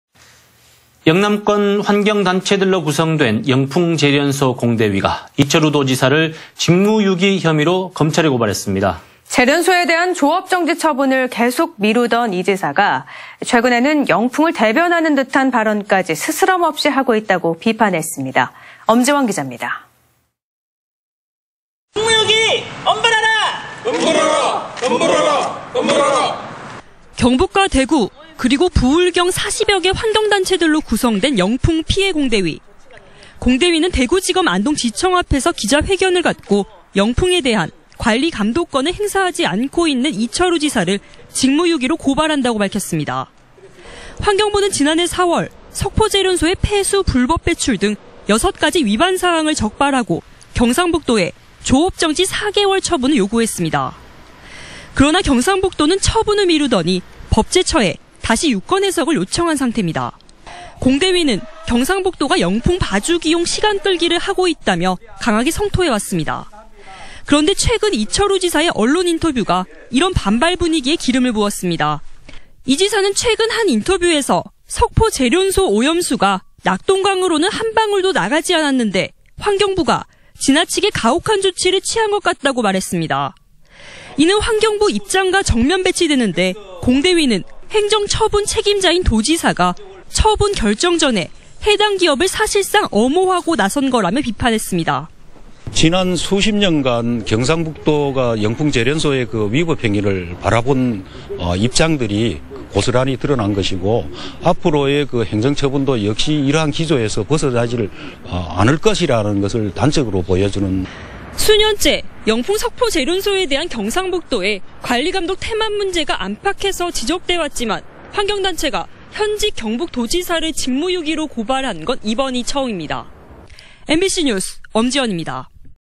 영풍 공대위, 이철우 도지사 '직무유기'로 고발 / 안동MBC
2020/02/14 15:16:20    작성자 : 엄지원

◀ＡＮＣ▶
영남권 환경단체들로 구성된
영풍제련소 공대위가, 이철우 도지사를
직무유기 혐의로 검찰에 고발했습니다.

제련소에 대한 조업정지 처분을
계속 미루던 이 지사가
최근에는 영풍을 대변하는 듯한 발언까지
스스럼없이 하고 있다고 비판했습니다.

엄지원 기자
◀ＥＮＤ▶
◀ＶＣＲ▶

◀ＳＹＮ▶
"직무유기 엄벌하라, 엄벌하라, 엄벌하라!"

경북과 대구, 그리고 부·울·경 40여 개
환경단체들로 구성된 영풍 피해 공대위.

공대위는 대구지검 안동지청 앞에서
기자회견을 갖고, 영풍에 대한
관리·감독권을 행사하지 않고 있는 이철우
지사를 직무유기로 고발한다고 밝혔습니다.

환경부는 지난해 4월,
석포제련소의 폐수 불법 배출 등
6가지 위반사항을 적발하고 경상북도에
조업정지 4개월 처분을 요구했습니다.

그러나 경상북도는 처분을 미루더니
법제처에 다시 유권해석을 요청한 상태입니다.

공대위는 경상북도가
'영풍 봐주기용 시간끌기'를 하고 있다며
강하게 성토해 왔습니다.

그런데 최근 이철우 지사의 언론 인터뷰가
이런 반발 분위기에 기름을 부었습니다.

[C.G] 이 지사는 최근 한 인터뷰에서
"석포제련소 오염수가 낙동강으로는
한 방울도 나가지 않았는데, 환경부가
지나치게 가혹한 조치를 취한 것 같다"고
말했습니다.[끝]

이는 환경부 입장과 정면 배치되는데,
공대위는 행정처분 책임자인 도지사가
처분 결정 전에 해당 기업을 사실상
엄호하고 나선 거라며 비판했습니다.

◀ＩＮＴ▶김수동 공동대표/영풍제련소 공대위
"지난 수 십 년간 경상북도가 영풍제련소의
위법 행위를 바라본 입장들이 고스란히 드러난
것이고 앞으로의 행정처분도 역시 이러한
기조에서 벗어나지를 않을 것이라는 것을
단적으로 보여주는.."

(S/U)"수 년째 영풍 석포제련소에 대한
경상북도의 관리·감독 태만 문제가 안팎에서
지적돼 왔지만, 환경단체가 현직 경북도지사를
검찰에 고발한 건 이번이 처음입니다"

MBC뉴스 엄지원입니다. (영상취재 원종락)